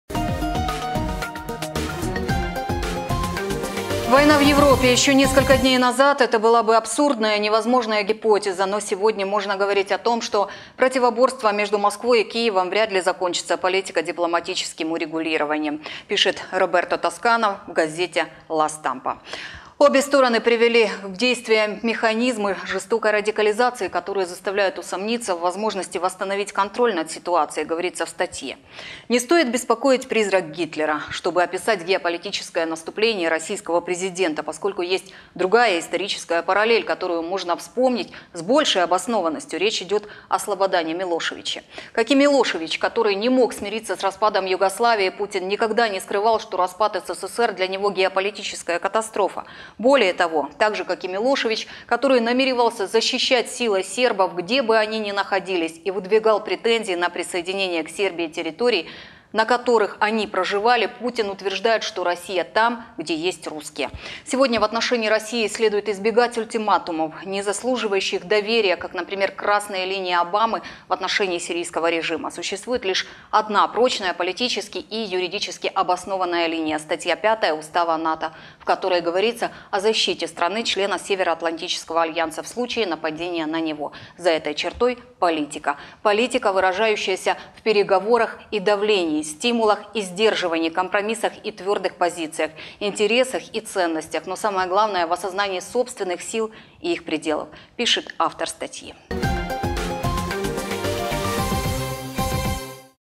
Путин идет по стопам Милошевича
Война в Европе? Еще несколько дней назад это была бы абсурдная, невозможная гипотеза, но сегодня можно говорить о том, что противоборство между Москвой и Киевом вряд ли закончится политико-дипломатическим урегулированием, пишет Роберто Тоскано.